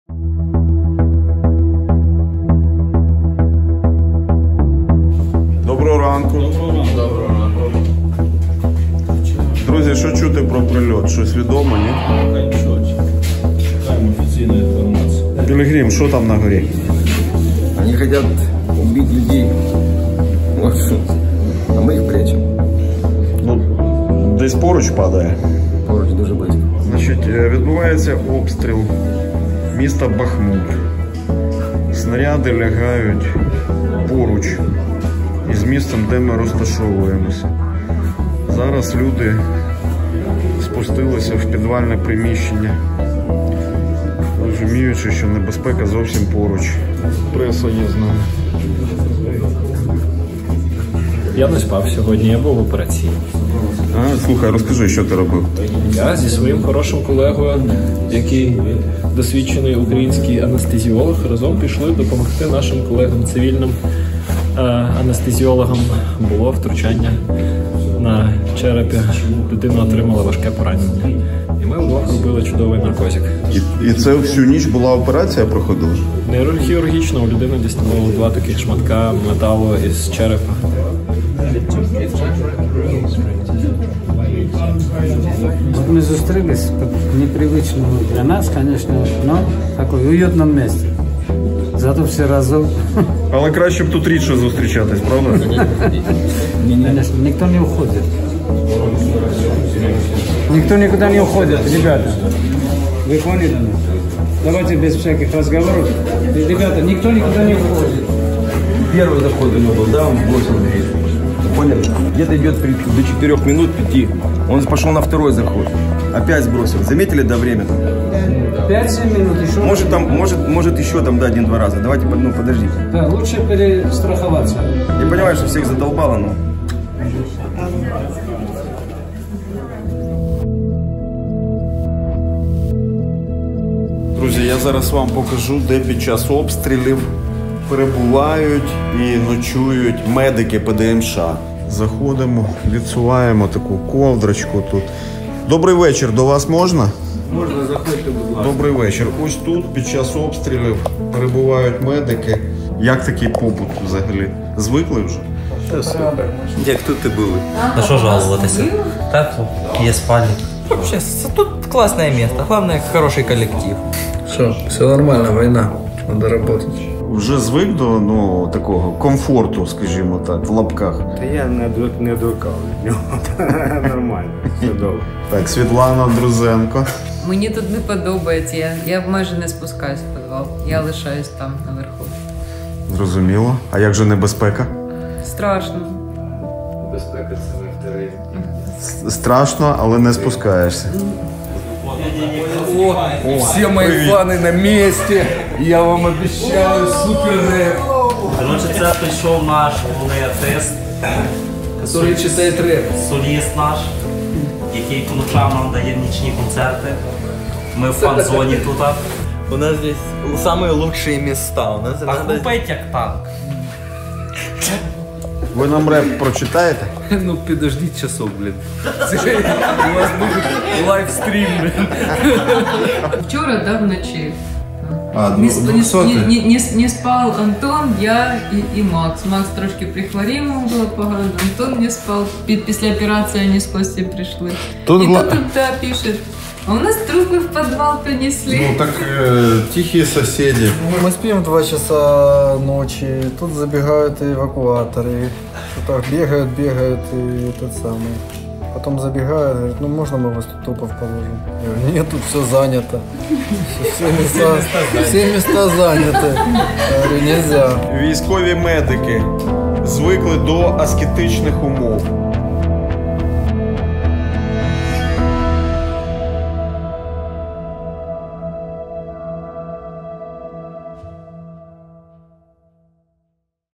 ЛІКАРІ ПІДЗЕМЕЛЛЯ
В Бахмуті медики-добровольці ПДМШ жили в трикутнику: швидкі-операційна-підвал лікарні. Російські бомби вибухали в кількастах метрах від шпиталю.

Але попри неймовірну втому, вони не припиняли жартувати та сміятись. Завдяки майстерній журналістській роботі Дмитра Тузова, ви можете на 10 хвилин зануритись у реалії великої війни. І згадати: в більшості міст України відносно тихо тільки тому, що хтось щомиті ризикує і жертвує своїм життям на Донбасі.